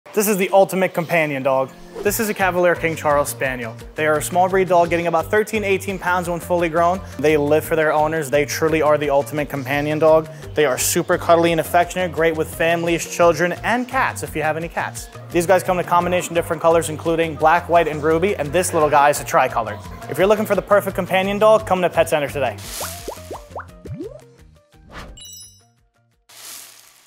The Ulitmate Companion Dog 🐶 ❣️
The Cavalier King Charles Spaniel is renowned as the ultimate companion puppy for several reasons. Their gentle and affectionate nature, combined with a friendly demeanor, makes them exceptional companions for individuals and families alike. These delightful pups thrive on human interaction, forming deep bonds with their owners, and their adaptable and sociable personality ensures they seamlessly integrate into various lifestyles. With their expressive eyes and loving disposition, Cavalier King Charles Spaniels bring joy, comfort, and unwavering companionship, solidifying their status as the ultimate canine companions.

🐶🩷Your new furry friend awaits – come in and make a connection that lasts a lifetime!

The Shoppes at Old Bridge
3833 US 9